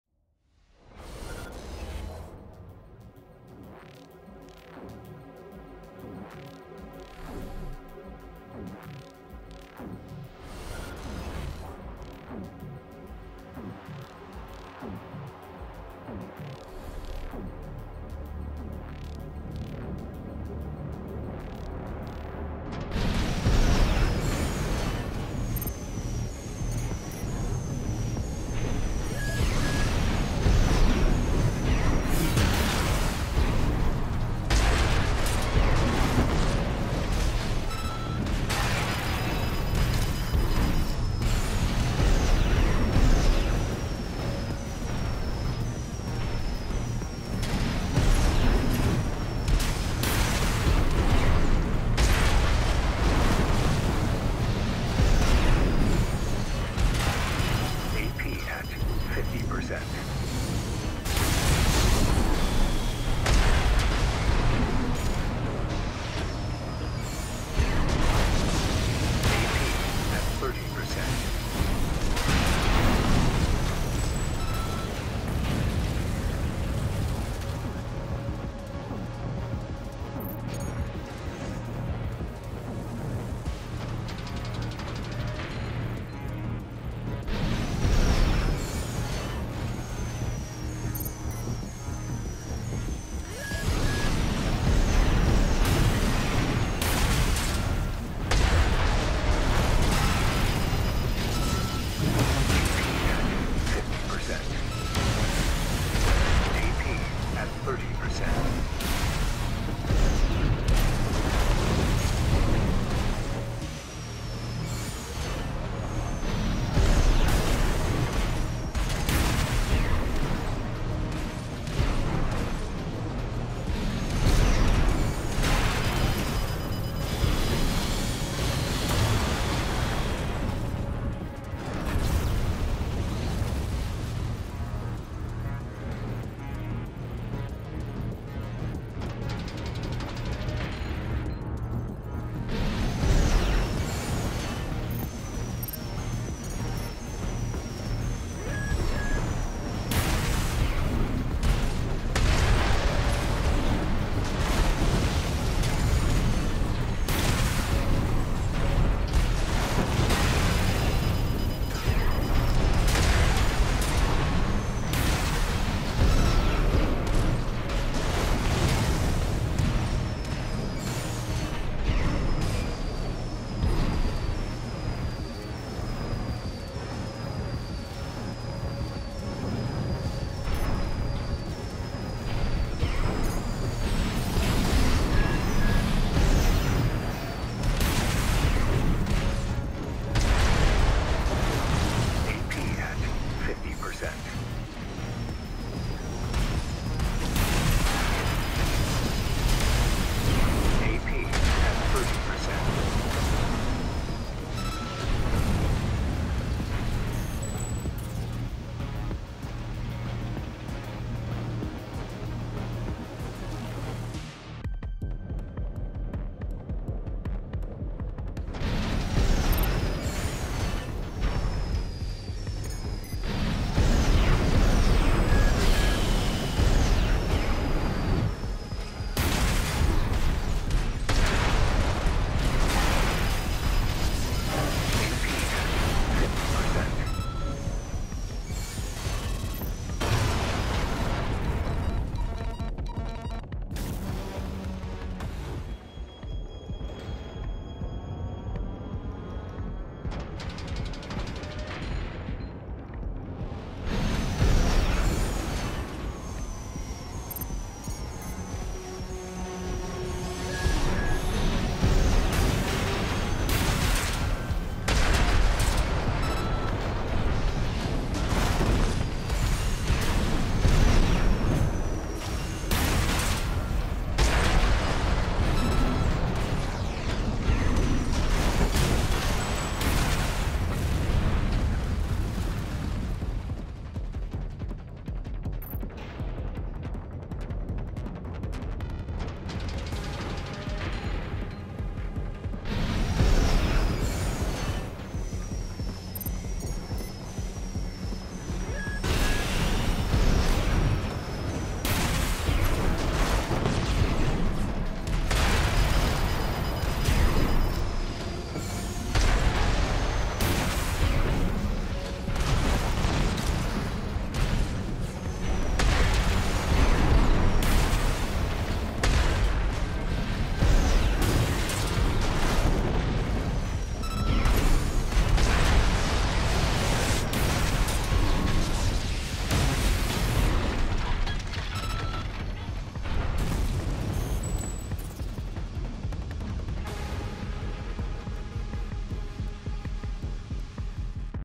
Every junkdog has its yard
This build is not finished yet, because I REALLY want to make the chainsaw work, and admittedly I am too spoiled off of the laser lance, and also on the Harris. I really like the Harris but I think it's too 'precise' of a weapon for the theme.

If the Dual Zinnerman users could stop just leaving after they win once so I can practice on new builds that would be nice too lmao.

Edit: IT'S CALLED THE JUNKYARD DOG WHY DIDN'T I MAKE IT A QUAD 5HEAD